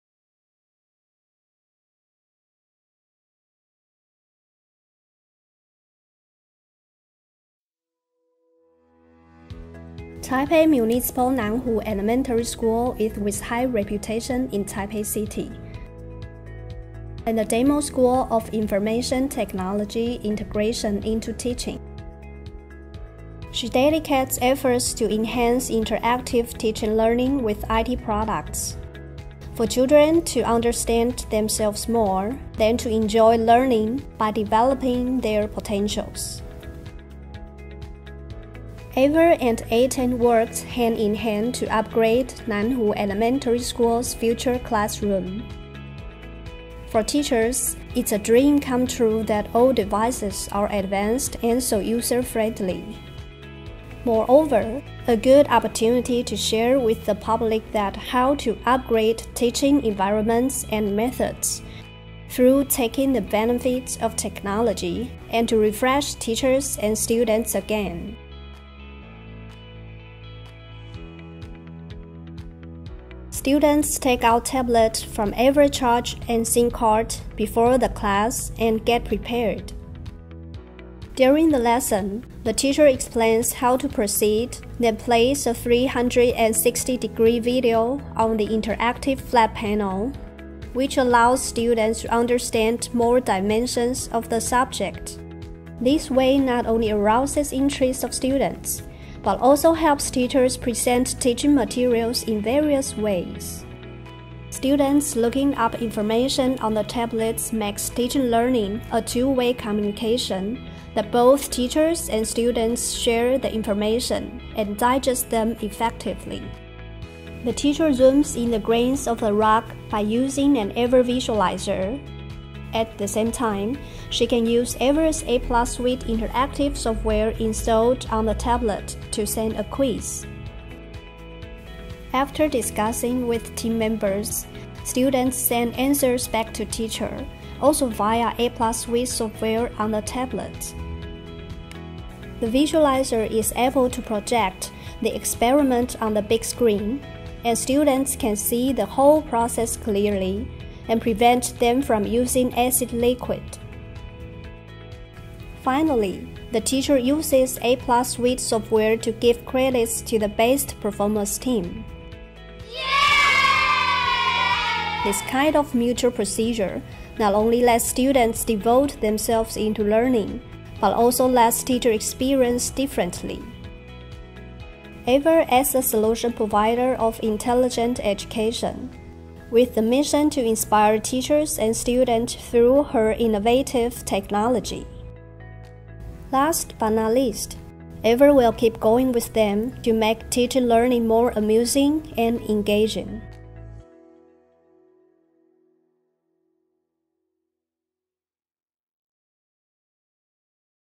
AVer Education Total Solution at Taipei Municipal Nan-Hu Elementary School
Taipei Municipal Nan-Hu Elementary School is with high reputation in Taipei city, and the demo school of Information Technology Integration into Teaching. AVer and ATEN worked hand in hand to upgrade Nan-Hu Elementary School's Future Classroom.